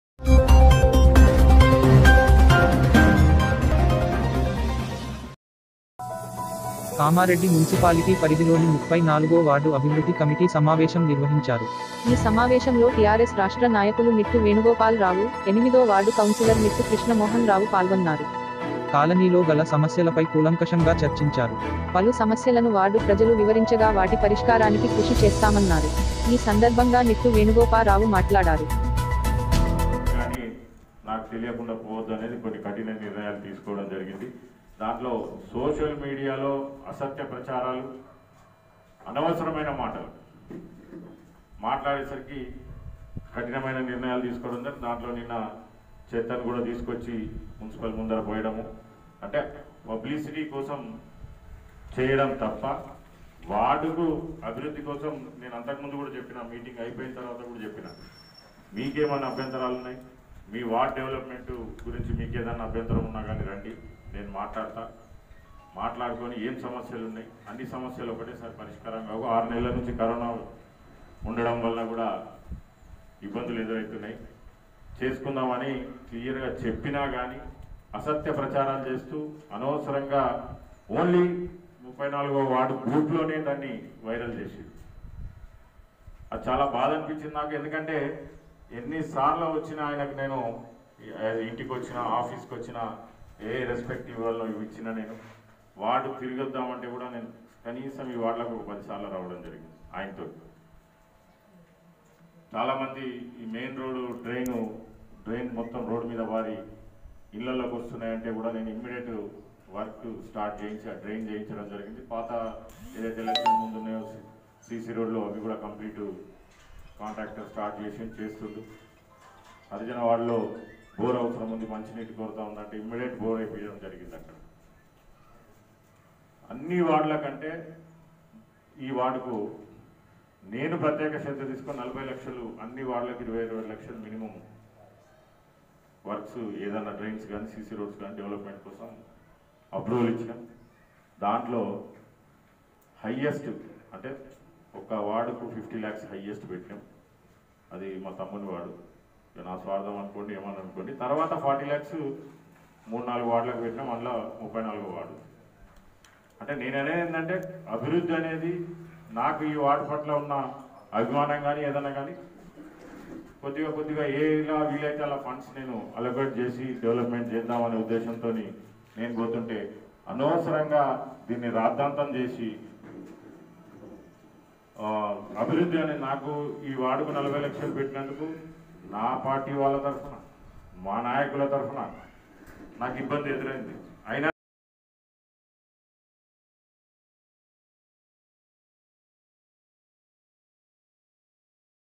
కామారెడ్డి మున్సిపాలిటీ పరిధిలోని 34 వ వార్డు అభివృద్ధి కమిటీ సమావేశం.
కామారెడ్డి మున్సిపాలిటీ పరిధిలోని 34 వ వార్డు అభివృద్ధి కమిటీ సమావేశం నిర్వహించారు. ఈ సమావేశంలో టిఆర్ఎస్ రాష్ట్ర నాయకులు నిట్టు వేణుగోపాల్ రావు, 8 వ వార్డు కౌన్సిలర్ నిట్టు కృష్ణమోహన్ రావు పాల్గొన్నారు. కాలనీలో గల సమస్యలపై కూలంకషంగా చర్చించారు. పలు సమస్యలను వార్డు ప్రజలు వివరించగా వాటి పరిష్కారానికి కృషి చేస్తామన్నారు. ఈ సందర్బంగా నిట్టు వేణుగోపా రావు మాట్లాడుతూ.. వార్డులో సమస్యలు పరిష్కరించడం లేదని ఆందోళన చేయడం సరికాదన్నారు. బీజేపీ కౌన్సిలర్లు అవాకులు, చెవాకులు మాట్లాడుతున్నారని అన్నారు. అన్ని వార్డు కౌన్సిలర్లకు మాదిరిగానే 34వ వార్డుకి కూడా నిధులు మంజూరు చేశామని తెలిపారు. నిధుల కేటాయింపులో ఎలాంటి పక్షపాతం లేదని స్పష్టం చేశారు. వార్డులో 40 లక్షల రూపాయలు అభివృద్ధి కోసం కేటాయించడం జరిగిందని తెలిపారు. ఈ వార్డును దత్తత తీసుకోవడం జరుగుతుందని, వార్డులో ఎలాంటి సమస్య ఉన్నా తమ దృష్టికి నేరుగా తీసుకువస్తే సమస్య పరిష్కారానికి కృషి చేస్తామని హామీ ఇచ్చారు. అభివృద్ధి విషయంలో రాజీపడే ప్రసక్తే లేదని స్పష్టం చేశారు. వార్డు కౌన్సిలర్ ఏ పార్టీ వారైనా అవసరాలకు తగినట్టుగా నిధుల కేటాయింపు ఉంటుందని తెలిపారు. అంతేకాని ఒకరికి ఎక్కువ, మరొకరికి తక్కువ నిధులు కేటాయిస్తూ పక్షపాతం ప్రదర్శించడం లేదన్నారు. ఈ విషయాన్ని వార్డు ప్రజలు గుర్తుంచుకోవాలని కోరారు. పురపాలక మంత్రి కేటీఆర్, ప్రభుత్వ విప్ గంప గోవర్దన్ సహకారంతో కామారెడ్డి పట్టణ అభివృద్ధికి మున్సిపల్ పాలకవర్గం కృషి చేసుంటే అభివృద్ధికి చైర్మన్ సహకారం అందించడం లేదని చెప్పడం సరికాదన్నారు. వార్డు ప్రజలు సమస్యలను తమ దృష్టికి తీసుకురావాలని కోరారు